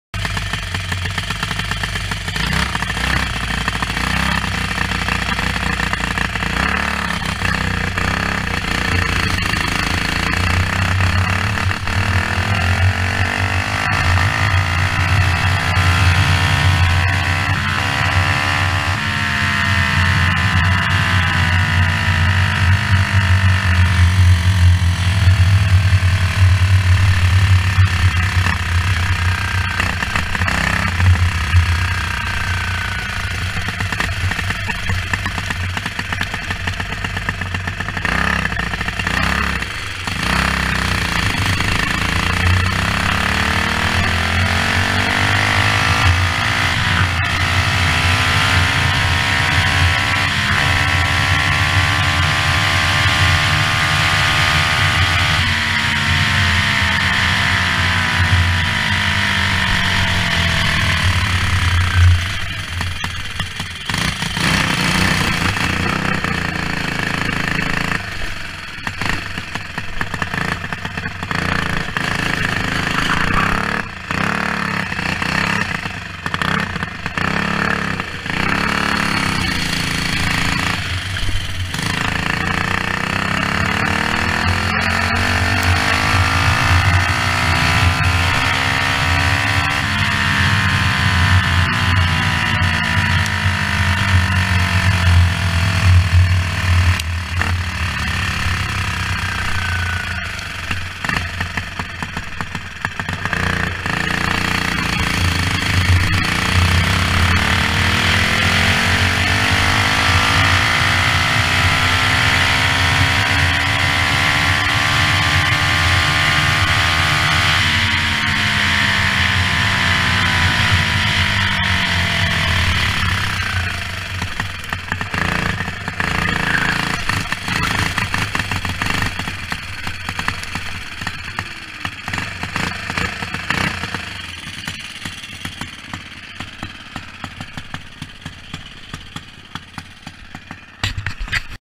AIRPORT GOKART RUN!!!
Testing out an open mod Animal engine 25+hp up and down the tarmac!!!